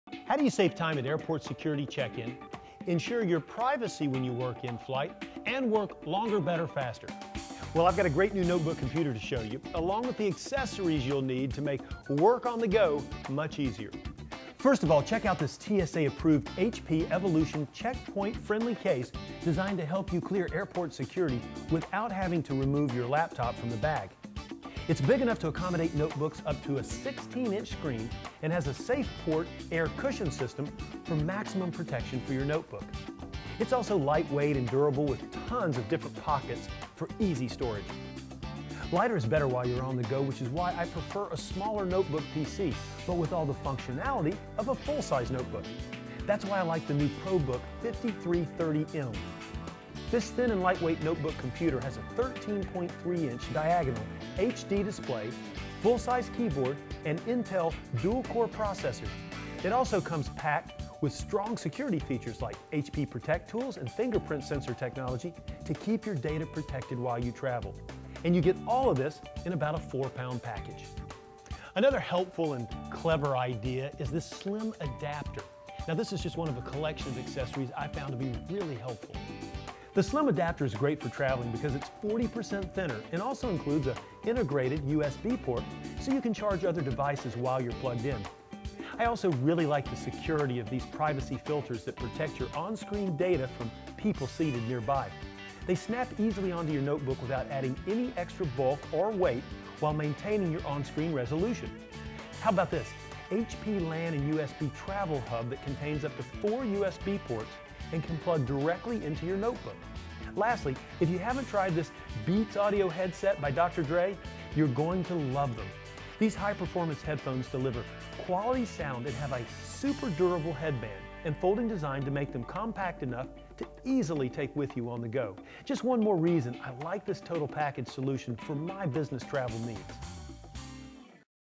Mark Bunting Takes A Look At The HP Probook 5330m & Some Great Travel Accessories - SkyTV
SkyTV's Mark Bunting takes a look at the HP Probook 5330m notebook computer and some great travel accessories to go along with it like the HP Evolution Checkpoint Frinedly Case, Slim Adapter, Privacy Filters, LAN & USB Travel Hub and Beats Audio Headset.